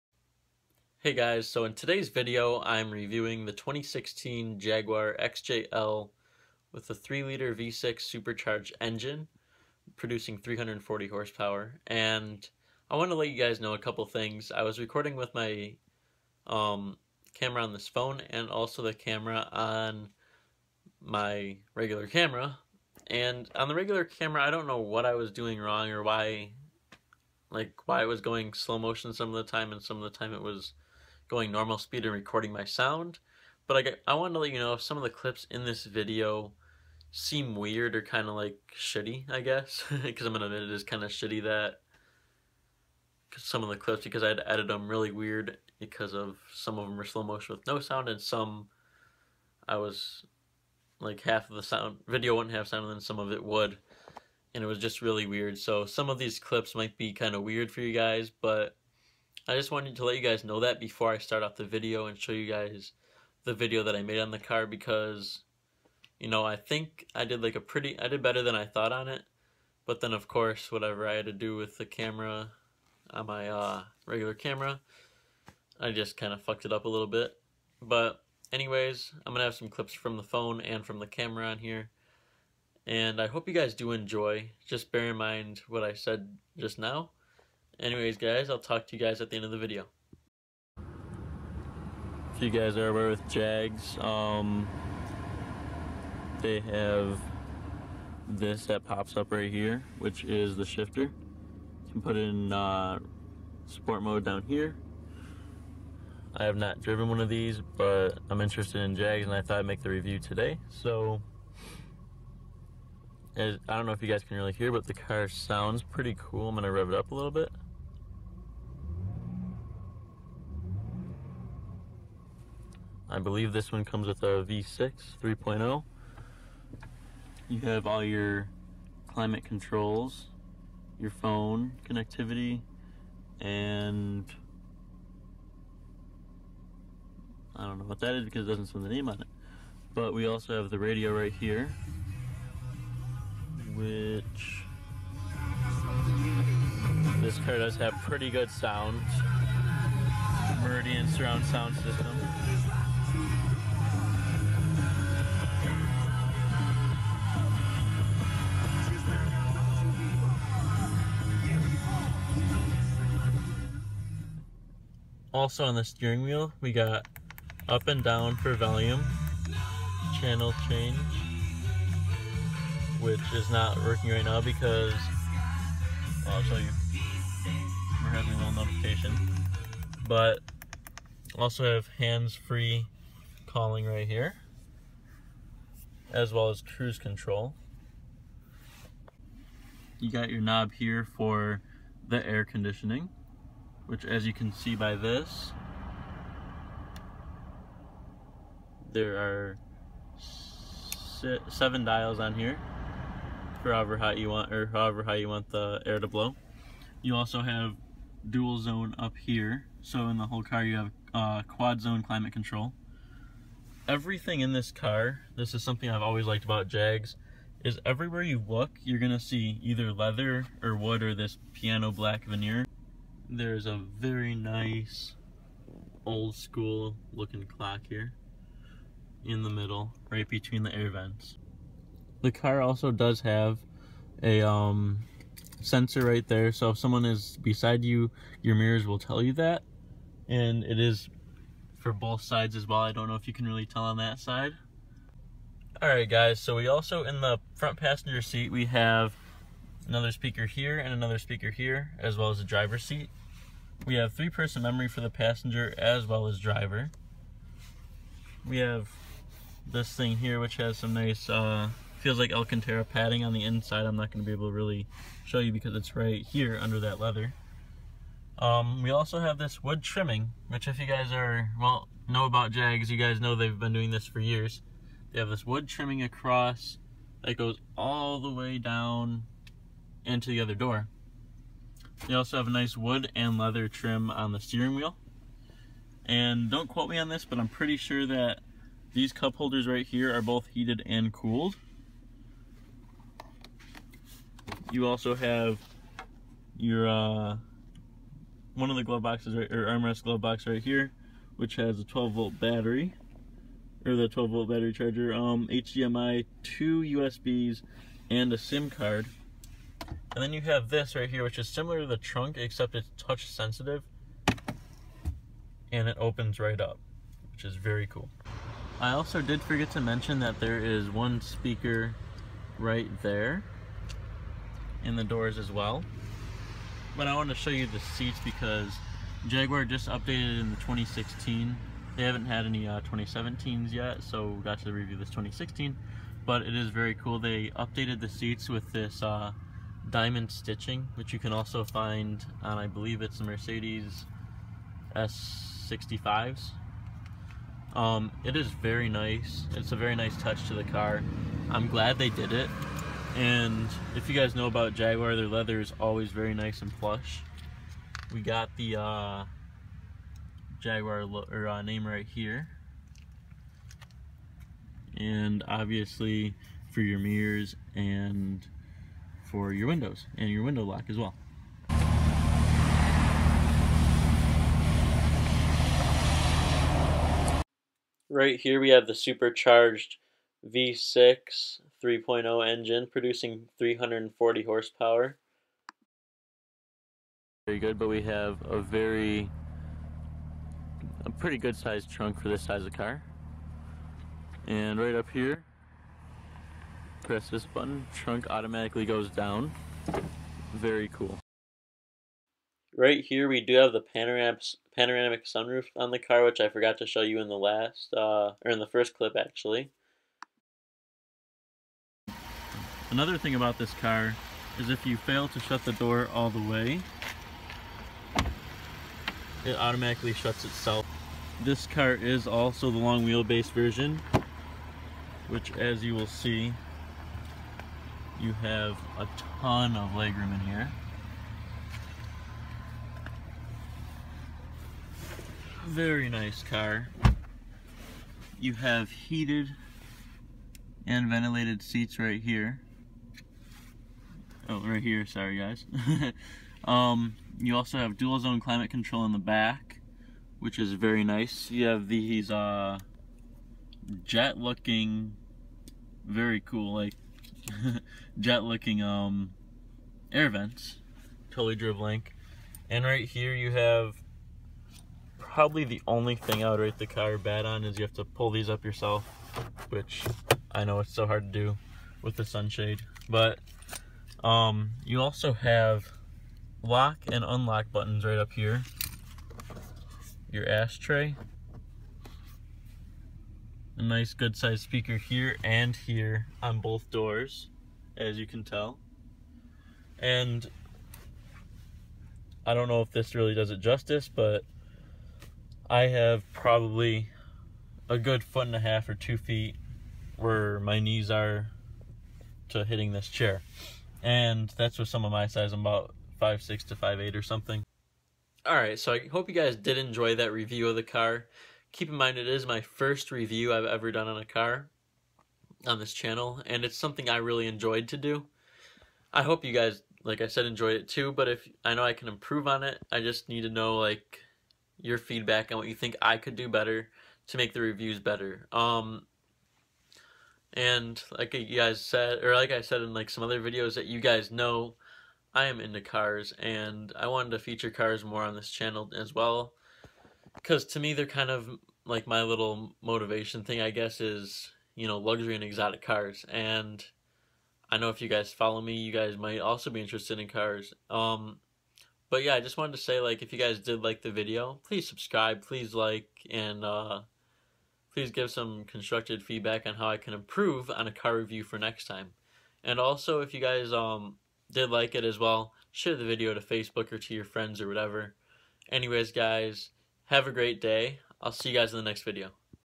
2016 Jaguar XJL Long Wheelbase Stunning Supercharged 3.0 V-6 Interior and Exterior Review
In todays video I review the 2016 Jaguar XJL with a 3.0 supercharge V-6 engine.  I Show you guys some of the interior and exterior, and the features within the interior and exterior, as well as the supercharge 3.0 V-6 engine.

If you guys like this video I will be sure to make more, as I love cars and making this video was a blast!